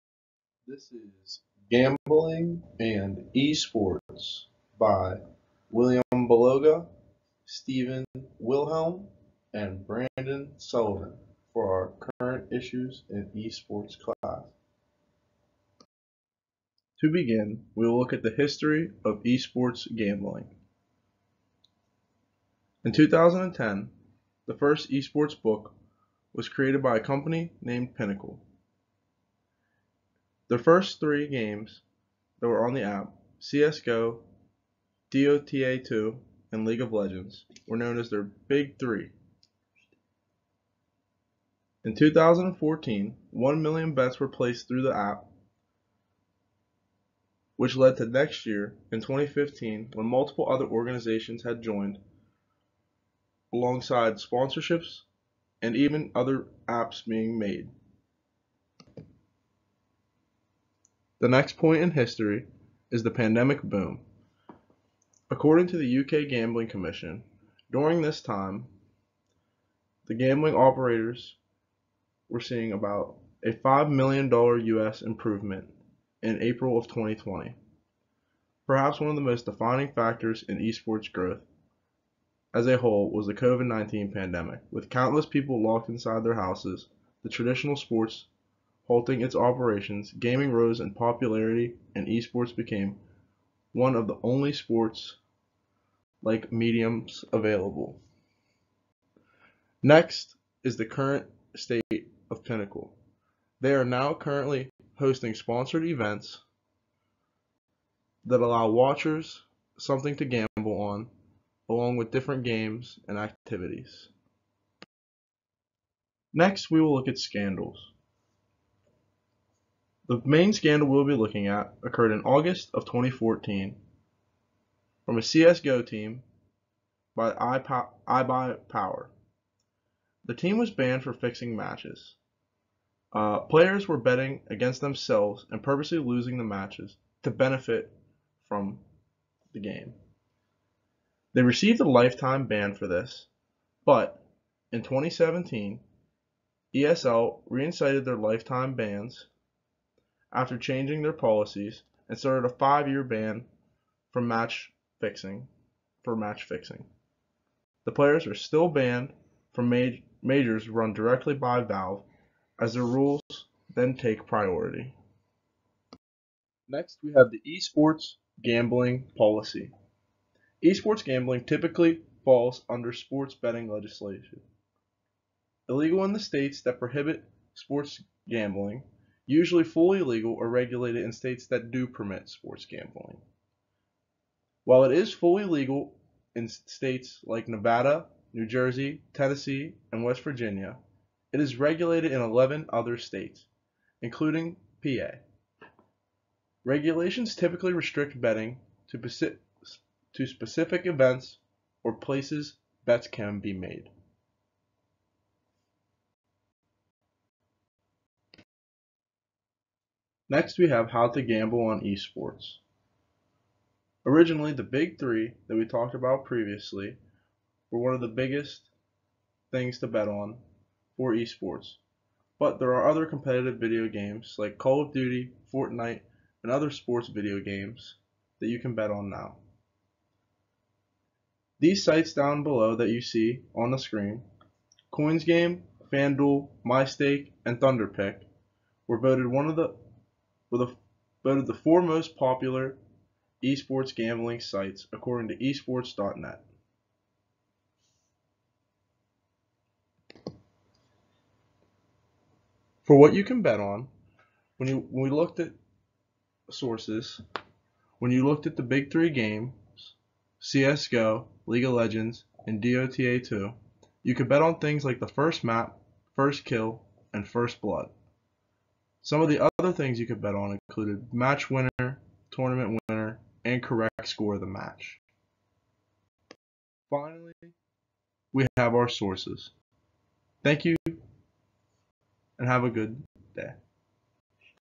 Gambling and Esports
by William Baloga, Brandon Sullivan, & Stephen Wilhelm

Gambling in Esports

This video project highlights the history, scandals, esports gambling platforms, and policy. One person mostly spoke during the recording while the other two focused on editing and creating the video and it's contents. The project itself derives facts and statistics from credible sources sourced at the end in APA format.

EXSC 304 - Current Issues in Esports Health & Society
Dr. Seth Jenny
Slippery Rock University of Pennsylvania